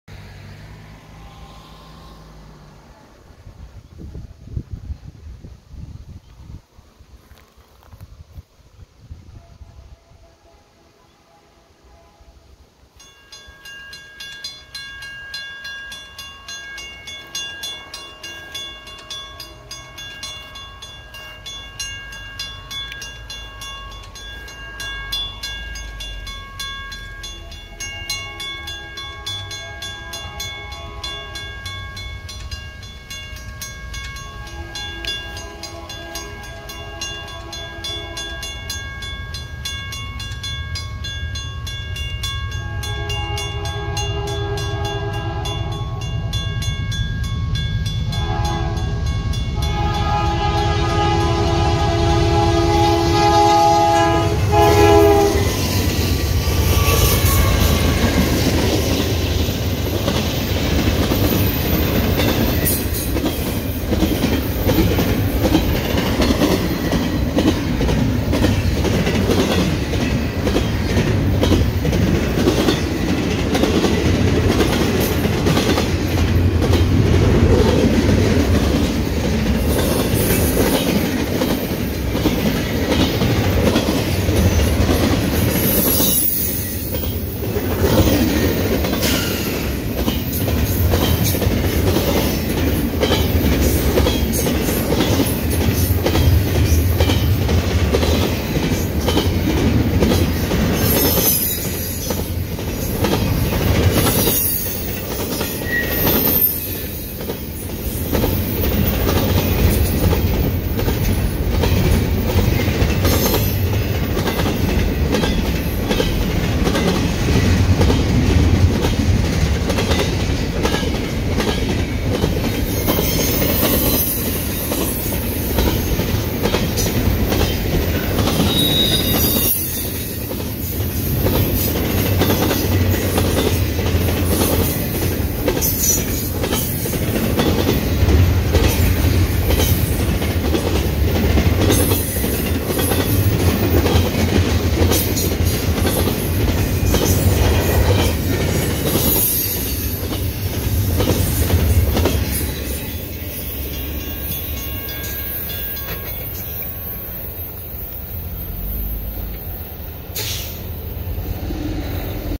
Mixed Bag Of Power On CN 120 With Dash 9 Leader!
Date: July 11, 2022
Location: Windsor Junction, NS
Train: CN Z120
Power: CN 2693 (C44-9W), CN 8890 (SD70M-2), CN 3944 (ES44AC), CN 4715 (GP38-2)